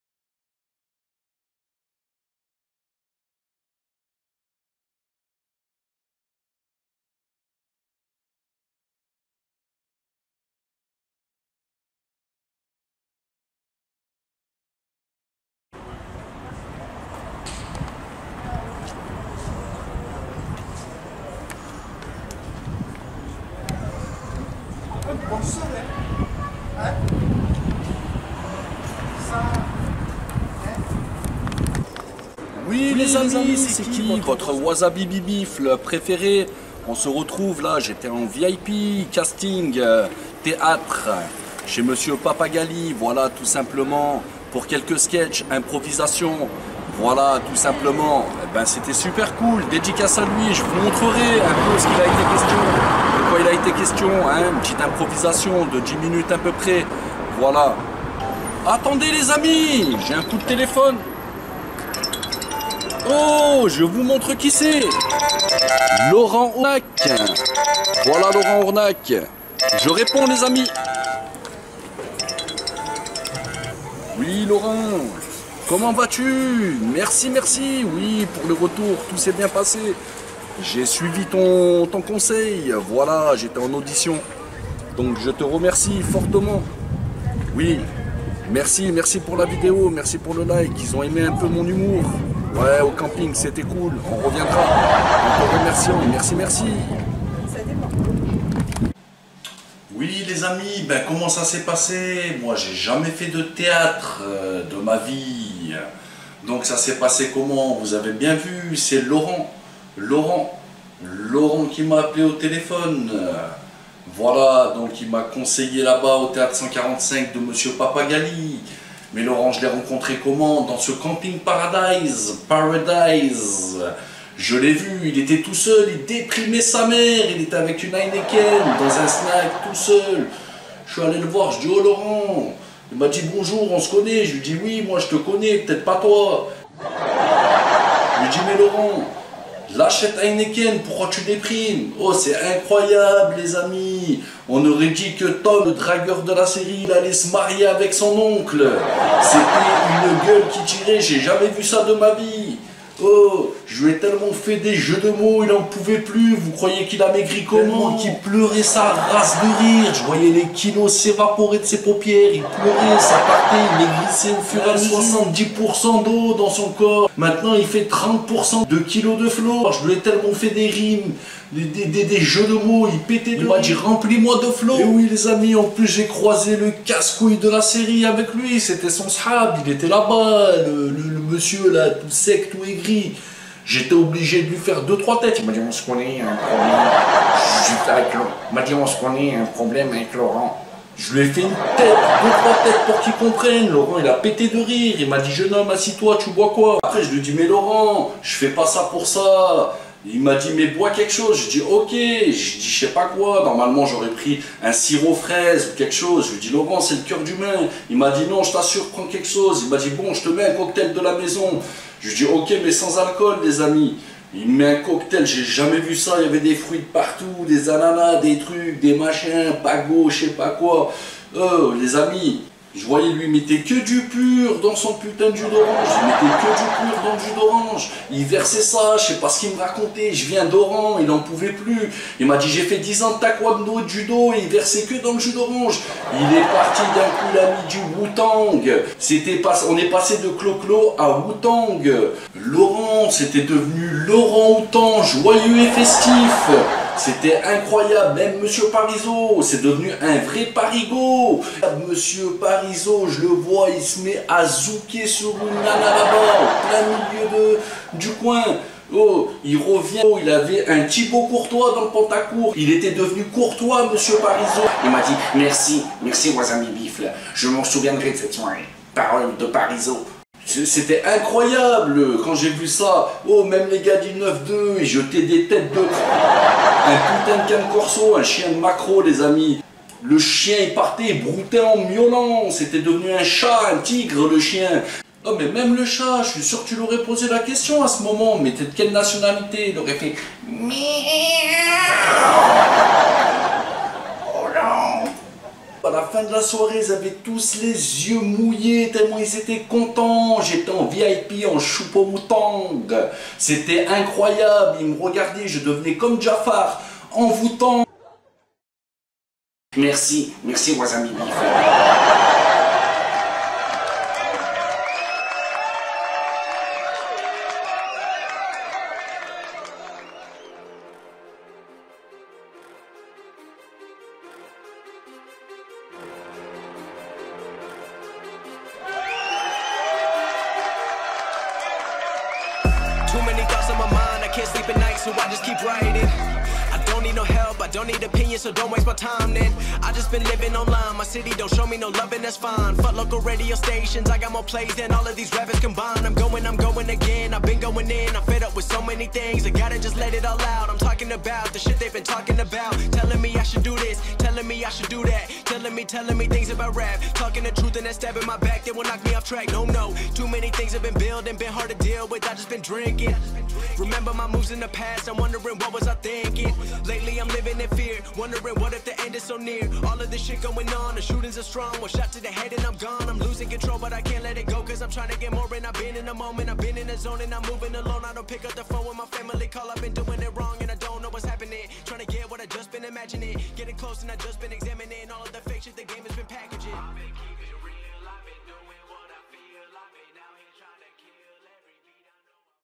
je passe un casting grace a laurent ournac
j explique la rencontre en vacance